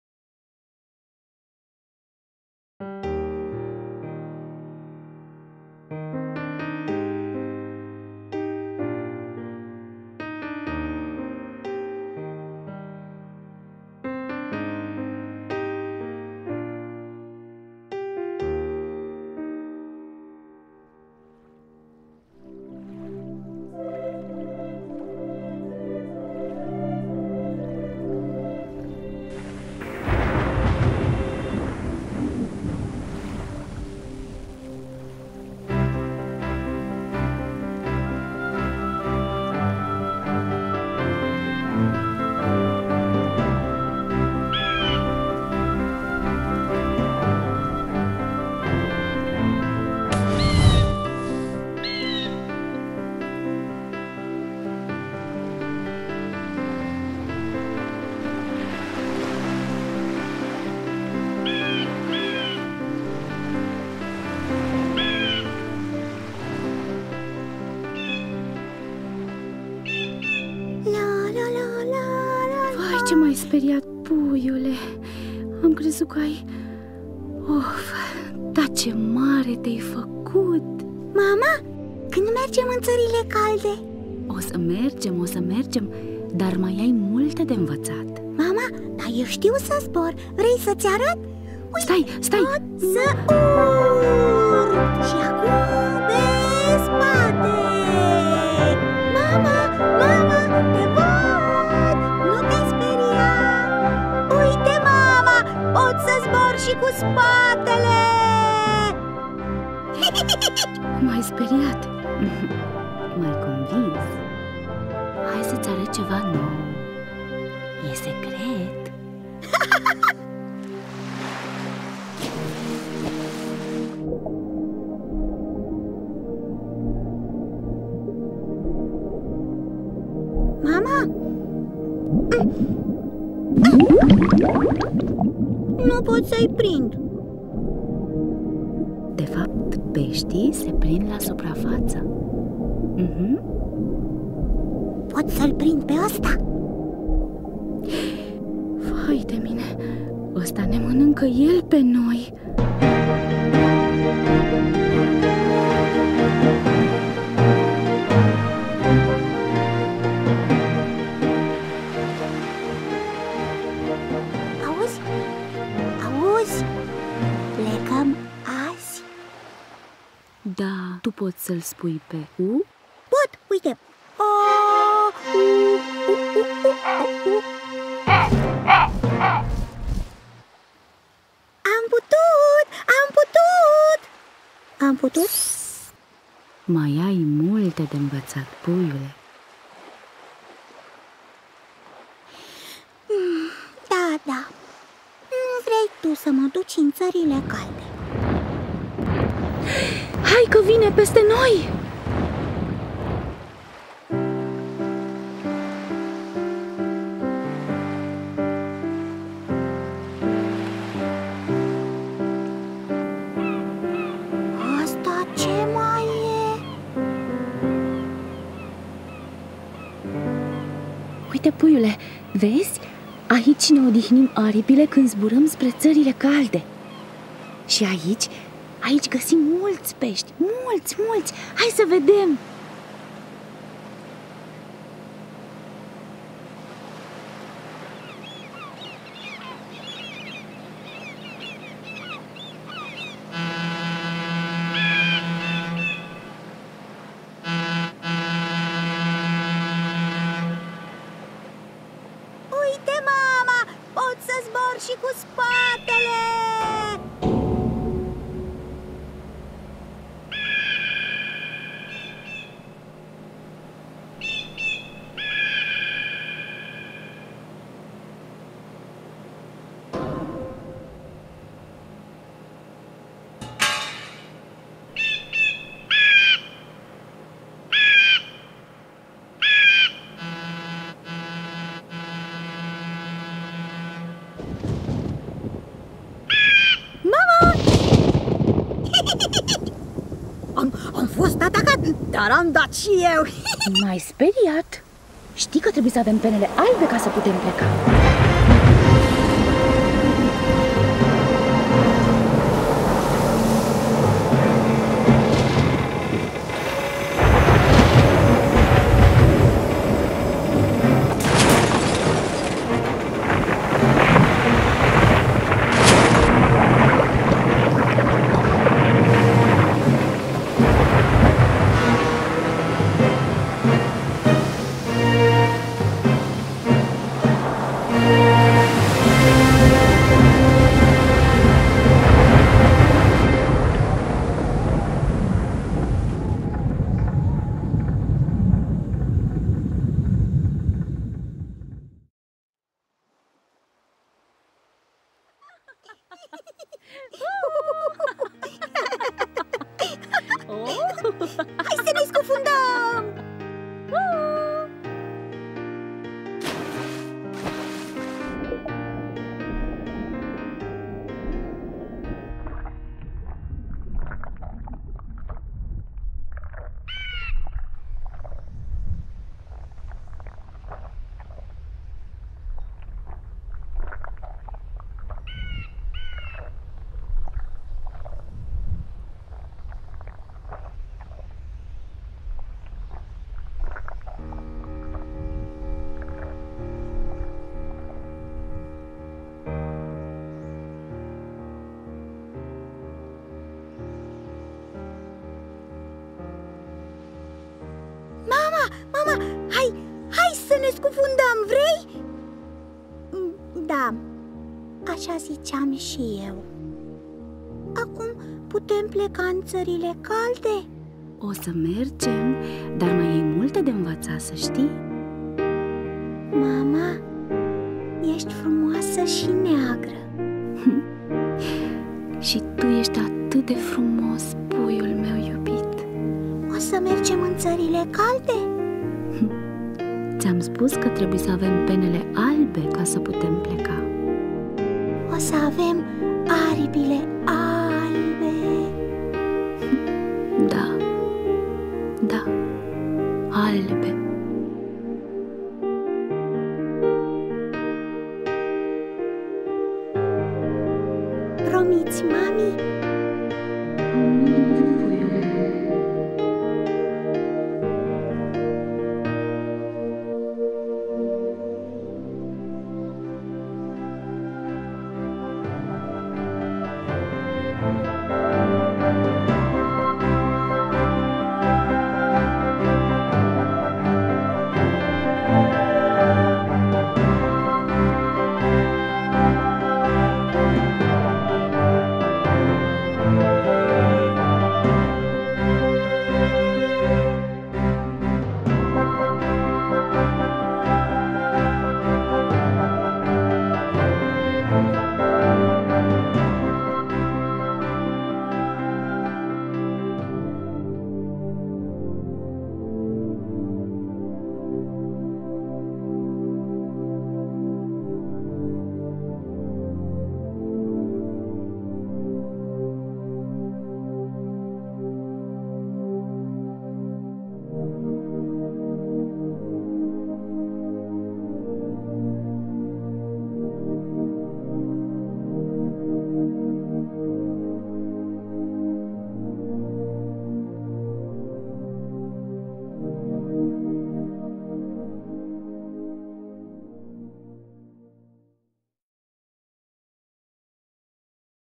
The song of innocence
Tragedia unor pasari acoperite de petrol.
Tragedy of oil covered birds